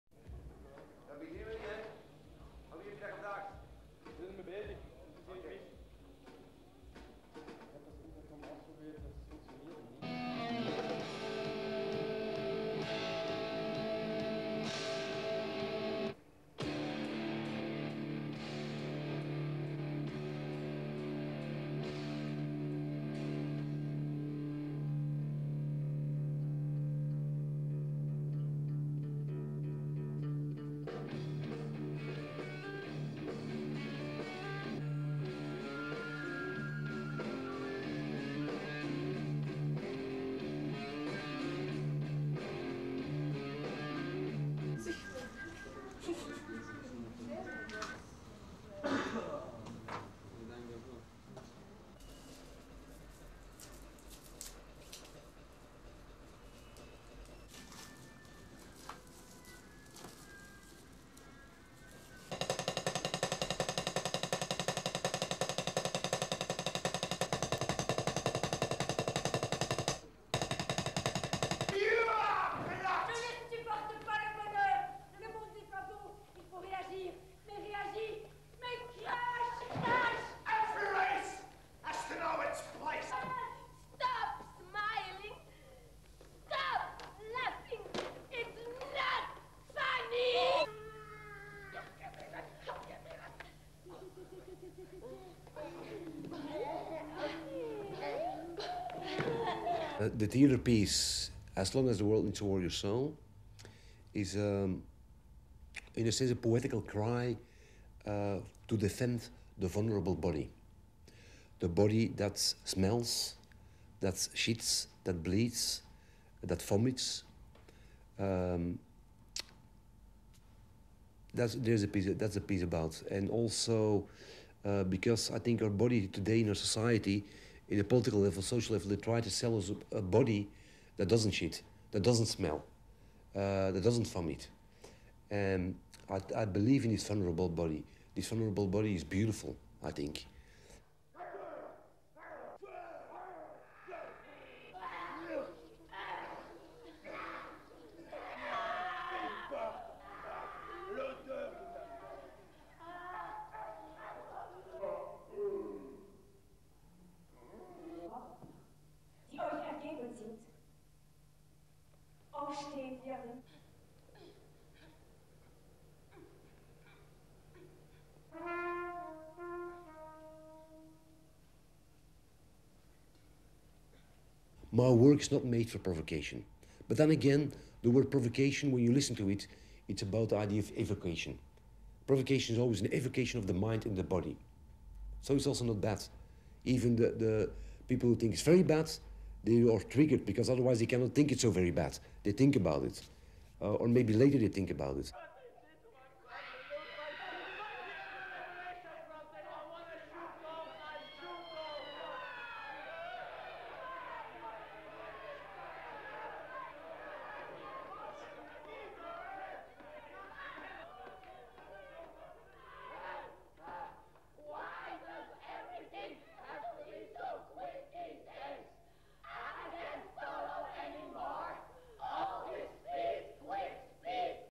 Belgium artist Jan Fabre works in Tel Aviv
(29 Mar 2001) SHOTLIST,TEL AVIV,24/03

1. Jan Fabre on scene surrounded by dolls
 2. Dancer warming up
 3. Scene with dolls
 4. Woman covering her body with cellophane
 5. Various of dancing
 6. Actress and dancer backstage before performance
 7. Dwarf covering his body with bacon
 8. Drummer warming up
 9. Various of performance : playing with dolls, woman crying, devil
covered with chocolate,...
 10. SOUNDBITE (English) of director Jan Fabre saying: " The theaterpiece As Long as the World needs a Warrior soul is a poetical cry to defend the vulnerable body. A body that smells, that shits, that bleeds, that vomits, that's the piece about. And also, because I think that our body today in the society in the political level and the social level, they are trying to sell us a body that doesn't shit, that doesn't smell, that doesn't vomit. I believe in this vulnerable body, this vulnerable body is beautiful I think".
11. Man and dwarf dancing covered with chocolate
12. Various of performance with dancers covered by chocolate
13. Actress putting a lump of butter into dancer's mouth
14. Ls performance
15. SOUNDBITE (English) of director Jan Fabre saying: " My work is not made for provocation but then again the word provocation, when you listen to it, it's about the idea of evocation, provocation is always an evocation of the mind through the body, so it's also not bad. Even the people who think that it is very bad, they are triggered because otherwise they cannot think that it is very bad, they think about it or maybe later they think about it."
16. More of dance and act performance

         BELGIAN DANCE GROUP SHOCK TEL AVIV

Belgian choreographer JAN FABRE's modern dance troupe have been in Israel this week, as part of a world tour of his latest work 'As long as a warrior needs a warrior's soul.'

16 dancers, actors and musicians perform the energetic and eclectic work, which is designed to shock.  It worked - the week-long show at the Tel Aviv Performing Arts Theatre which ended on Saturday has received a mixed reception.

Some members of the audience walked out in disgust after the first few minutes - but those that stayed to the end gave the troupe a standing ovation.

Full-frontal nudity and dancers smeared in a variety of foodstuffs - including chocolate spread, flour, eggs and ketchup - were just some of the ingredients that proved too strong for some members of the audience to swallow.

Dolls are also used to present a warrior taking on different shapes, as men and women in various situations and of different ages.

The central theme is body in revolt and characters in the work include a racist, an AIDS victim, a prostitute and a 40-year-old mother seperated from her husband.

The image of doll versus the human body is an underlying feature of the performance.  Characters want to become dolls themselves, to free themselves of the burdens of the body and its functions.  They project their wishes and desires onto the doll.

At the same time, there is resistance to the doll, as dancers use their bodies to express individuality, opposing dehumanisation.

The story is based on a Dario Fo text, which includes selections from the memoirs of Ulrike Meinhof of the Red Army faction's Beider-Meinhof Gang. The work's soundtrack jumps from hard rock to Billie Holiday numbers.

Fabre attempts to present a world of complete chaos where everyone tries to rule.  He is known for creating total, multi-disiplinary art and demands complete devotion and virtuoso abilities from his dancers and actors.